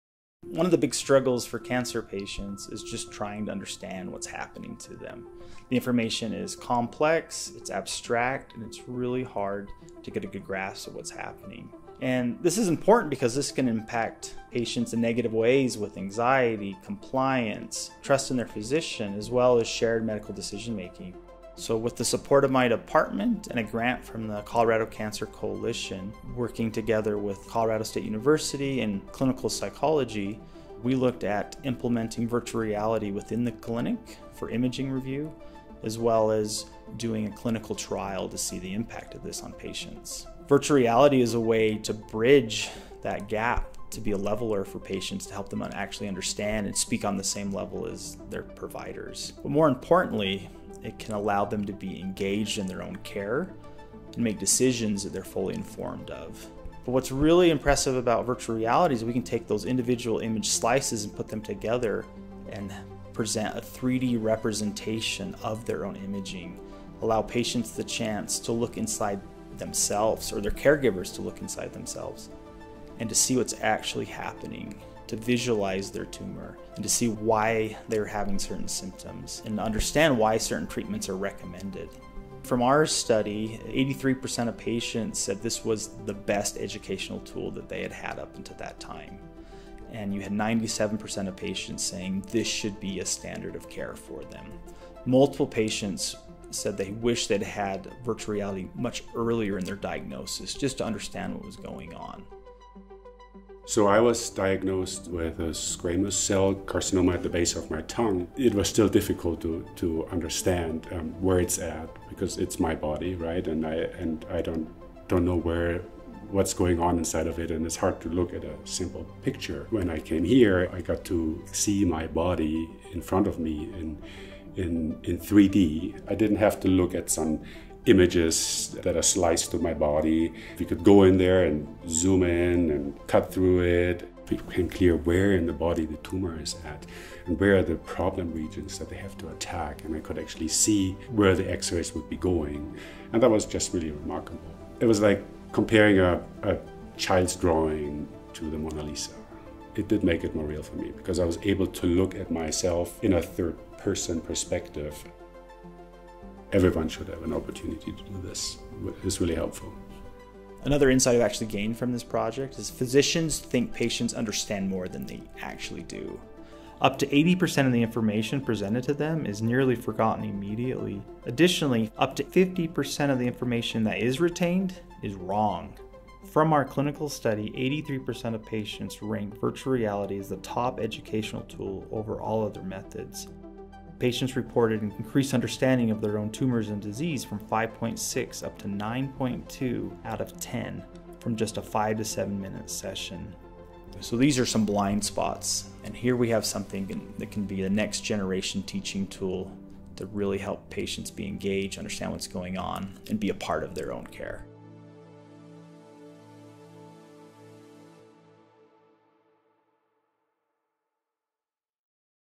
Virtual Reality as an Educational Tool for Cancer Patients
Douglas Holt, MD, developed an award-winning virtual reality pilot program that brings patients face to face with their tumors. While that may sound frightening, patients at the CU Cancer Center gave overwhelmingly positive feedback.

They said the VR sessions helped them better understand their condition, made them more likely to adhere to treatment regimens and helped them to ask better questions and be a part of their own care.